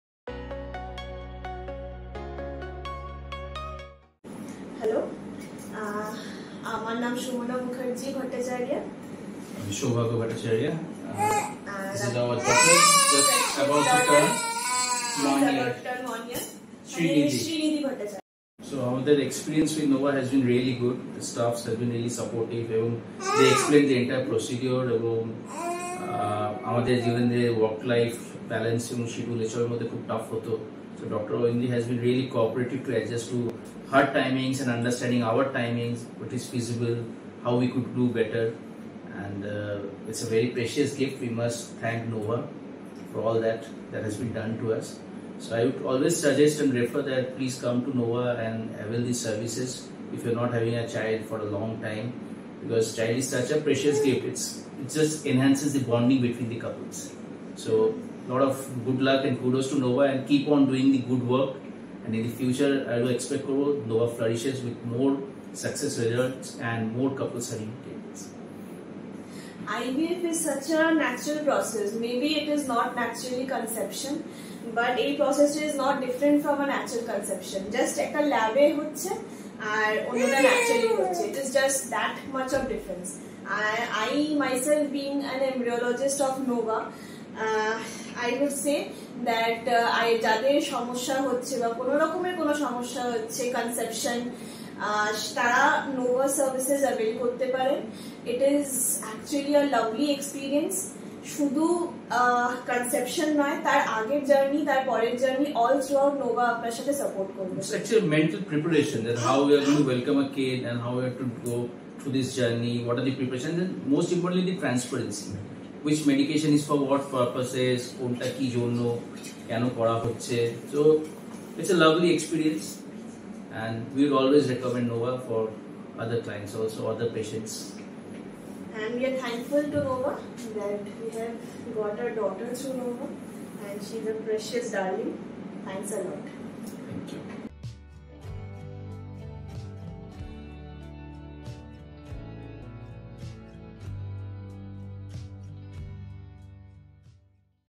The couple conceived after IVF treatment by Dr. Aindri Sanyal, IVF specialist, Nova IVF Kolkata
The couple took treatment from Dr. Aindri Sanyal, IVF specialist, Nova IVF Kolkata, and got a positive result of their treatment. They are very satisfied with the atmosphere of the clinic, the staff members, the doctor, and the services they received from Nova IVF.